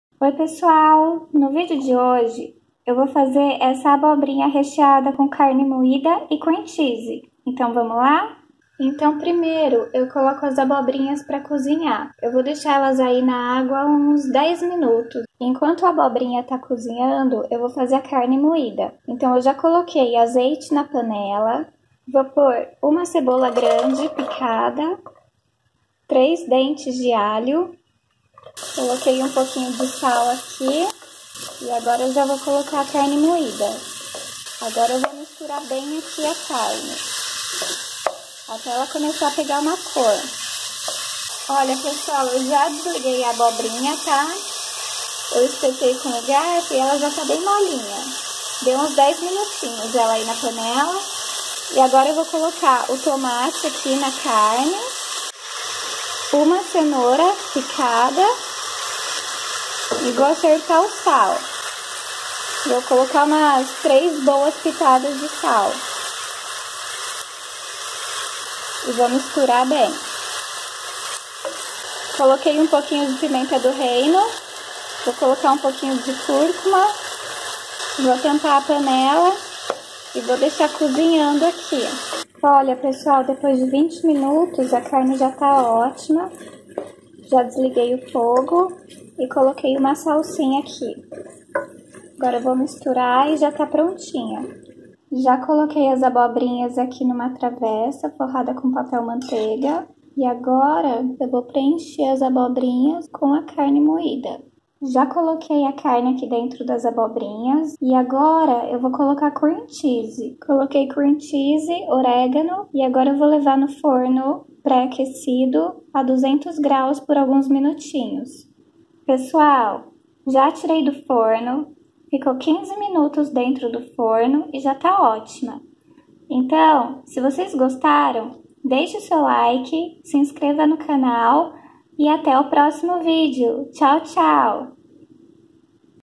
Abobrinha com Carne Moida e Cream Cheese - Receita Low Carb - Deliciosa e Fácil
Olá Pessoal!!!

Segue a Receita de Abobrinha com Carne Moida e Cream Cheese:

Ingredientes:

- 3 abobrinhas;
- 250gr de carne moida;
- 3 dentes de alho
- 1 cebola;
- 1 tomate;
- 1 cenoura;
- Salsinha;
- Cream Cheese;
- Oregano;
- Sal;
- Azeite;

Modo de Fazer:

Coloque as abobrinhas na água para cozinhar por cerca de 10min;
Em outra panela, coloque o azeite, refogue o alho, a cebola e a carne, assim que a carne dourar um pouco, coloque o tomate e a cenoura, acerte o sal, misture bem e tampe a panela; deixe cozinhar por uns 15min;
Ao ver que a carne está boa, desligue o fogo e coloque a salsinha, misture bem;
Em um refratário, corte as abobrinhas e retire a parte de dentro dela com uma colher, preencha as abobrinhas com a carne moida e por cima, coloque cream cheese e oregano;
Leve ao forno pre aquecido a 200 graus por uns minutos (até ver que o cream cheese esquentou), então, pode retirar do forno e está pronta!!

Obrigada e tenham um bom dia!!